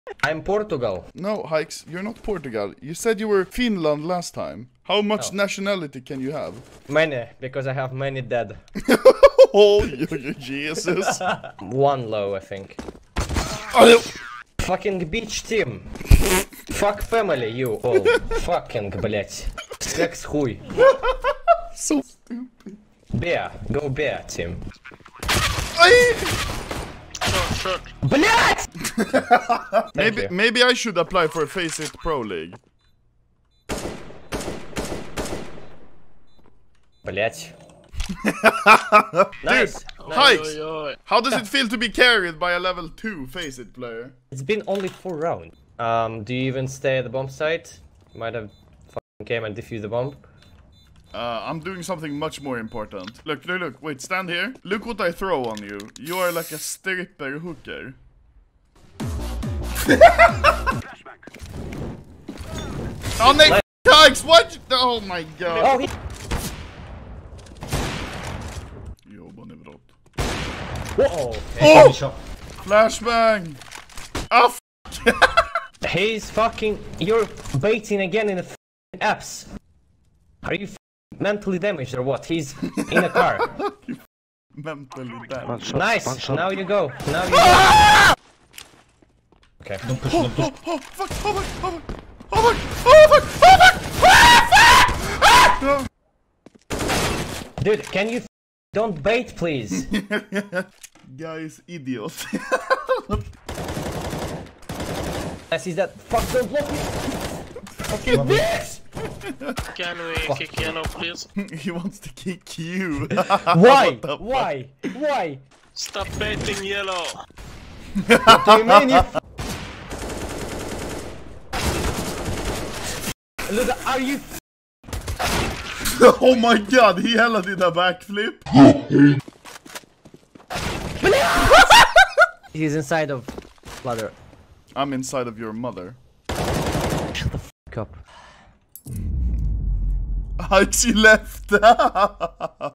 HAIX RAGED SO HARD DURING THESE GAMES!
Haix needs to chill out while he plays!

Shoutout the clip team: Samz, Akmega, Chodeslayer, Mald & Infr :-)